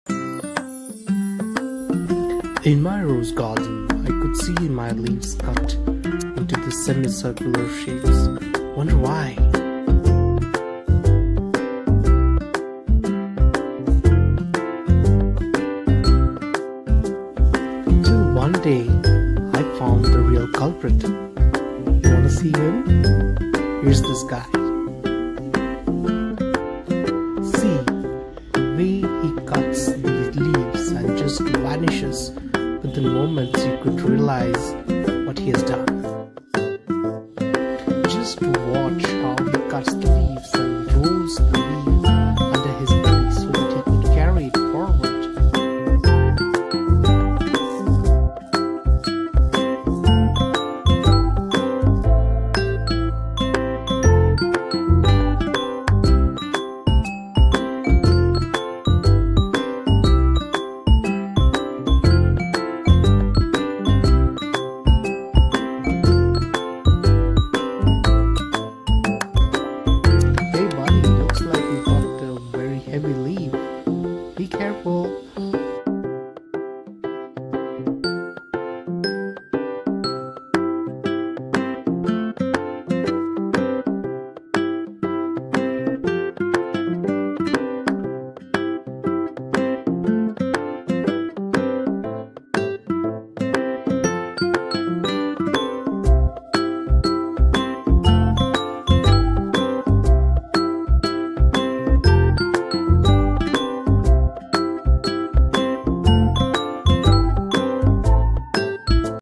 Mysterious leaf cutter bee.
watch this video which shows how this bee perfectly cuts leaf into semi circle and carry the portion of the leaf.